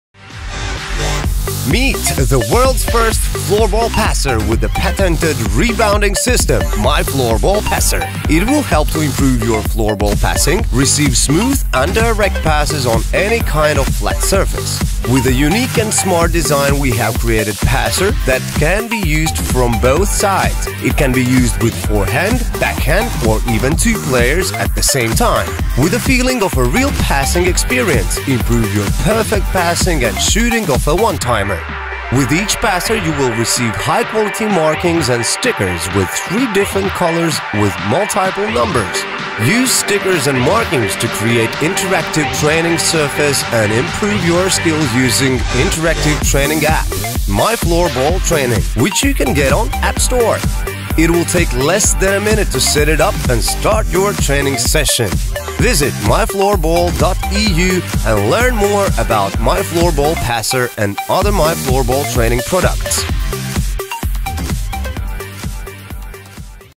My Floorball Passer | Florbal4u.com
MyFloorball Passer je prvý florbalový nahrávač s patentovaným odrazovým systémom "My Floorball Passer".

➡ Nahrávač Vám pomôže zdokonaliť Vašu florbalovú prihrávku, príjem loptičky a presnosť nahrávok na rovnom povrchu.
➡ Unikátny a elegantný dizajn nahrávača možno využiť z oboch strán.
➡ Možno teda trénovať forhandové aj backhandové nahrávky alebo možno nahrávač využívať súčasne u dvoch hráčov.
➡ Vďaka nahrávaču, ktorý vracia nahrávky reálnou rýchlosťou, je možné zlepšiť prihrávky a strely z "prvej".
➡ Ku každému nahrávaču dostanete vysokokvalitné nálepky a značky s tromi rôznymi farbami a číslami. Nálepky a označenia slúžia k interaktívnemu tréningu pomocou aplikácie my Floorball Training (dostupná v App Store a Google Play).